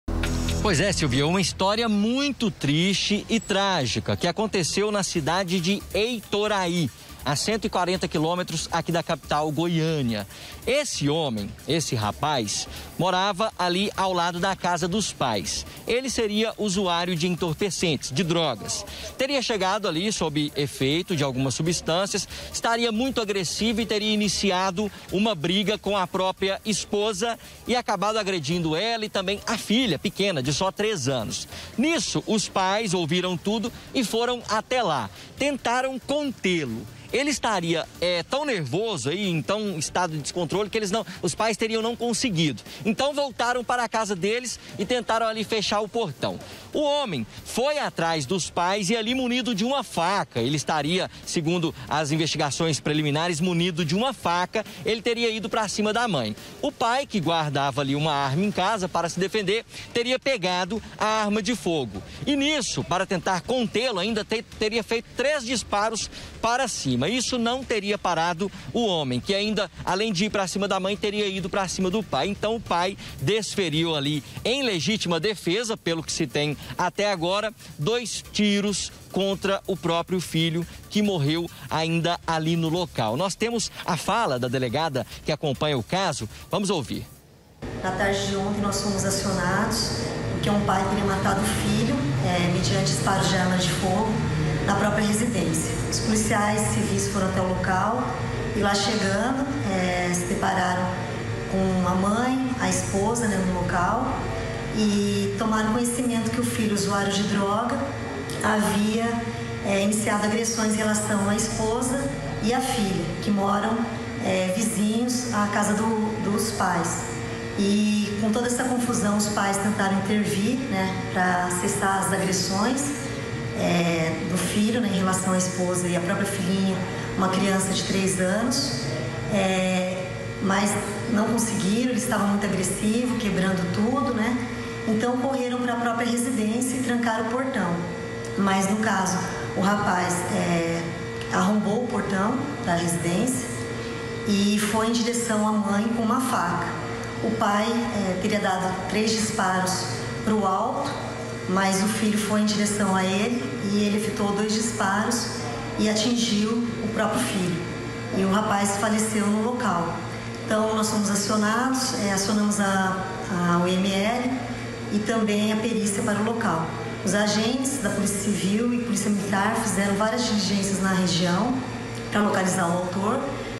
TRAGÉDIA EM FAMÍLIA: PAI MATA FILHO QUE AMEAÇAVA A PRÓPRIA MÃE
TRAGÉDIA EM FAMÍLIA. UM HOMEM MATOU O FILHO PRA EVITAR QUE O RAPAZ TIRASSE A VIDA DA PRÓPRIA MÃE. QUEM TEM OS DETALHES É O REPÓRTER DOUGLAS BRANQUINHO.